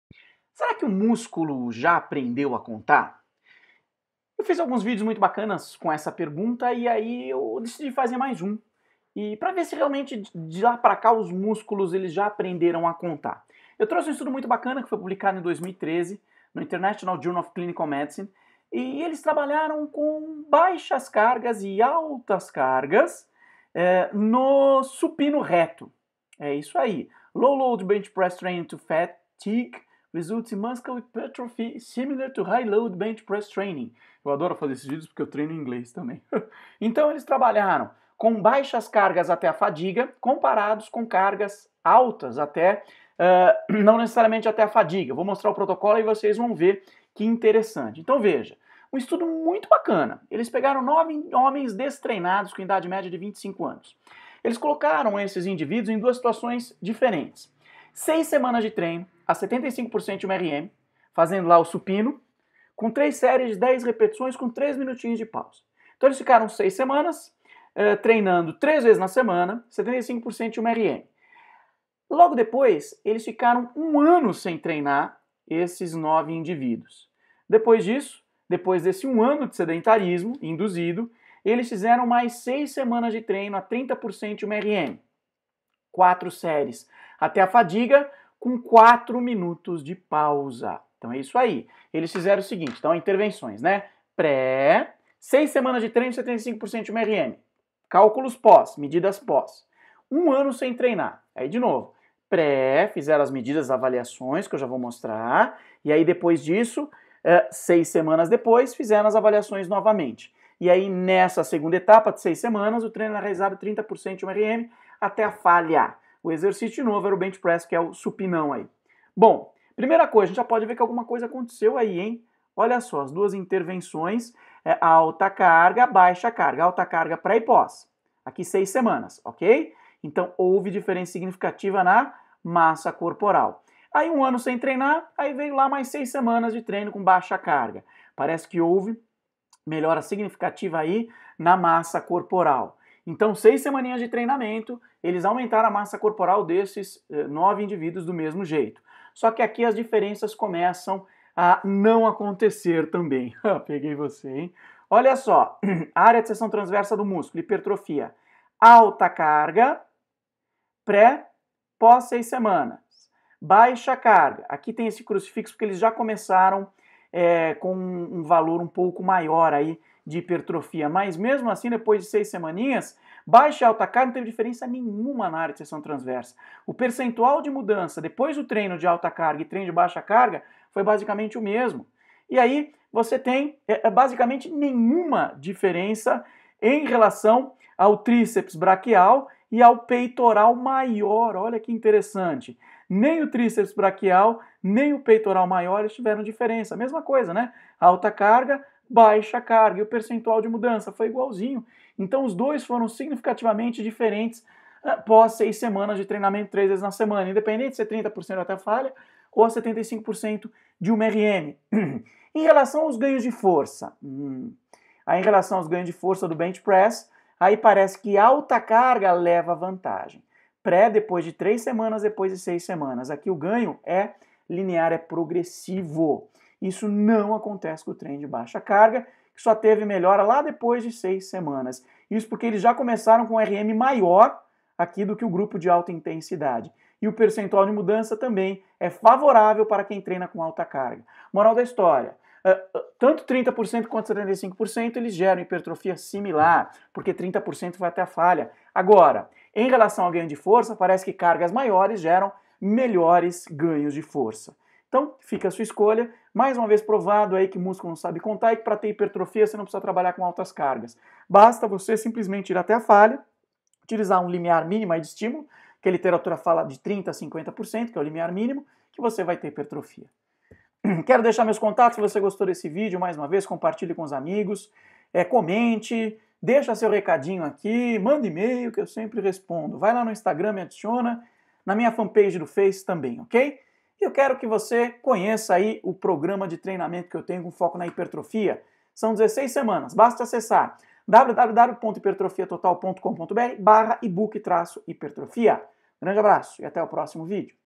O musculo ja aprendeu a contar ?
Será que o músculo já aprendeu a contar em um treino com foco na hipertrofia ?